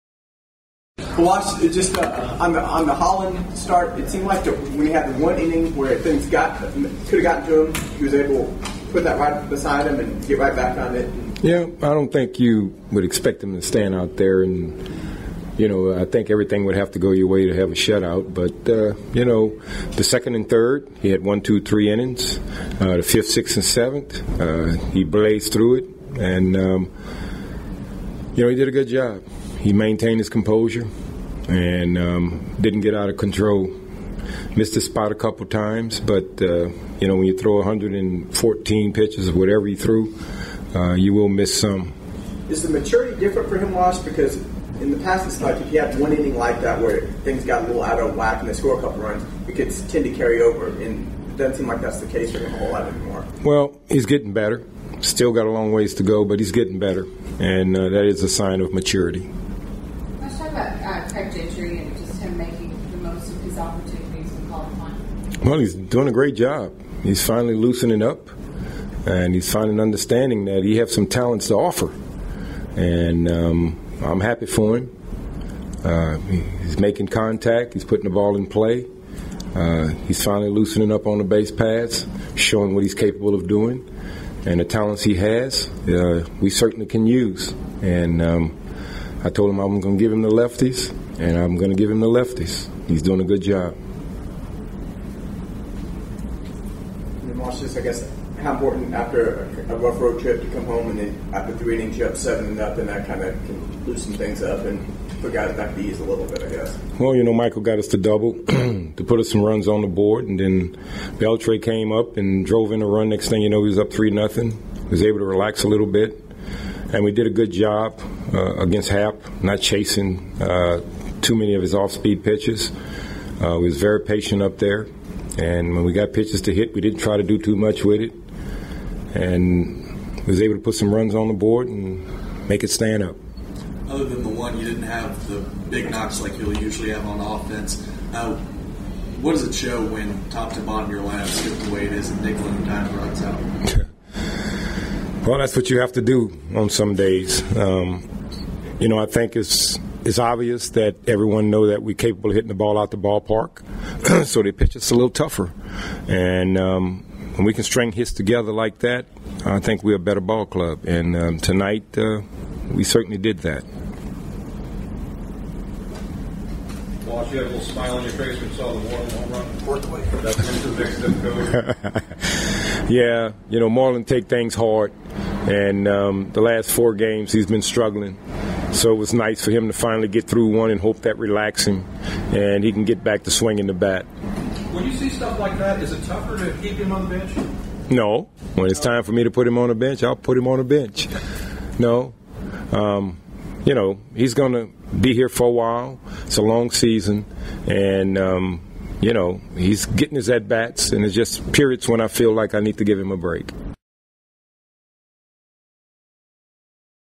Manager Ron Washington Post Game 06/20/11
Texas Rangers Manager Ron Washington talked to the media after the win over the Houston Astros on 06/20/11.